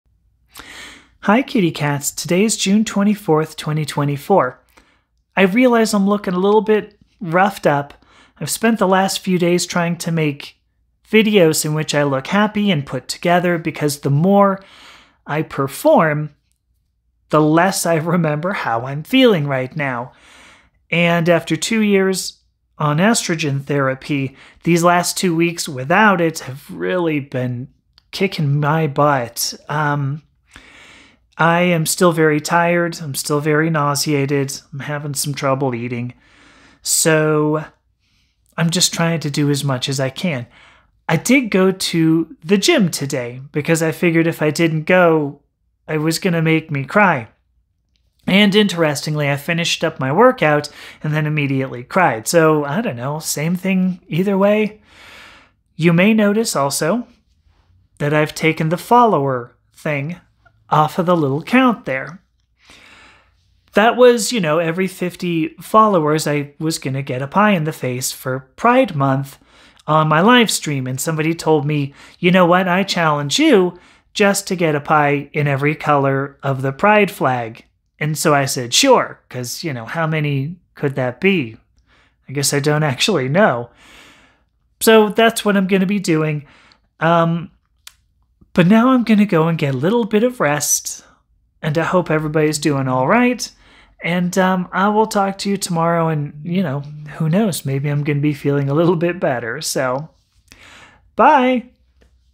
Even Counting Is Difficult without My Hormone Therapy!
Today I'm tired and need some rest after discontinuing HormoneTherapy before GenderAffirmingSurgery. I didn't even get the date right!

So instead, I'll try to figure out how many colors there are in the Pride flag for this coming weekend.